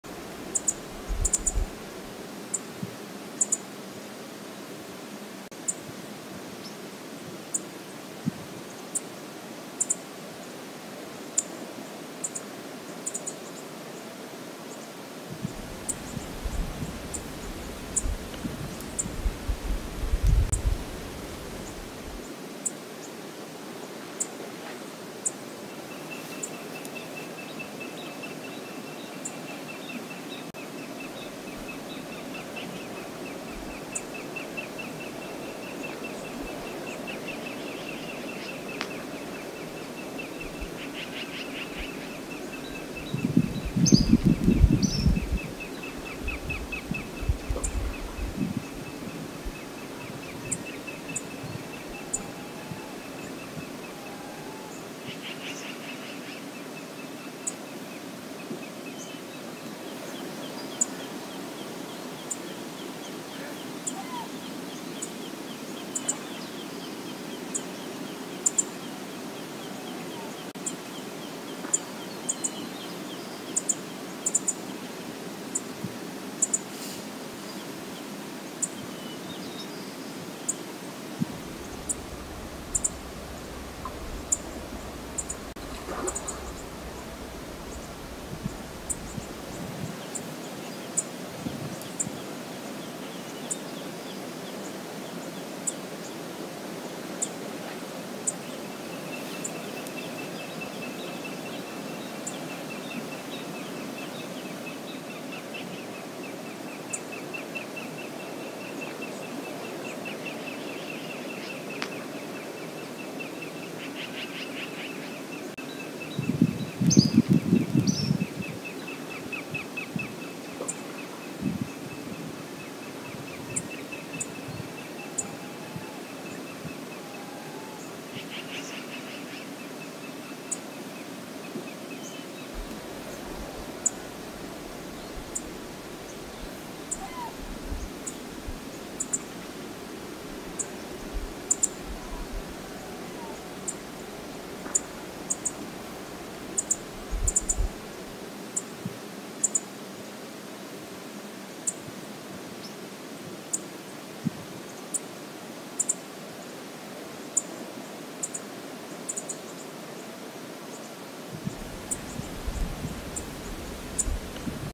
146 and ? on the Spit - July 10, 2023 (explore.org)
These are the two bears that were playing while maybe 335 was out on the spit this morning. The lighter bear looks like 146 aka "Addie" (see mckate's playlist for her The darker bear kind of reminds me of 344, but I'm not sure.